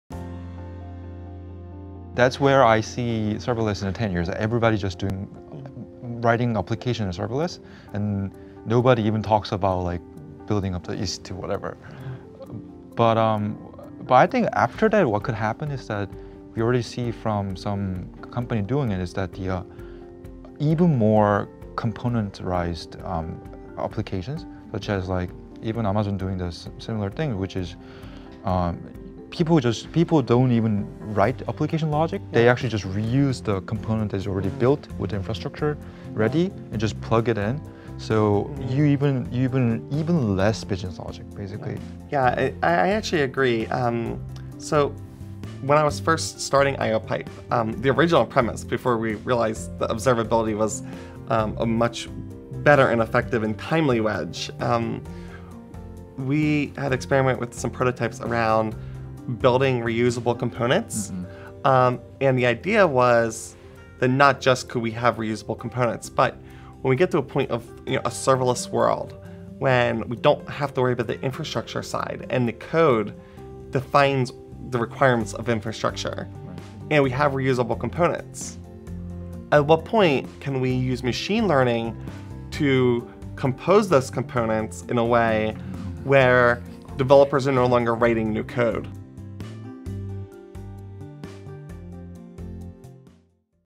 Where I See Serverless in Ten Years
Kurt Lee, AWS Serverless Hero and Tech Lead at Vingle, sees the future of serverless as having "even more componentized applications...people won't even have to write application logic, they'll actually just be able to reuse a component that's already built with the infrastructure ready and just plug it in."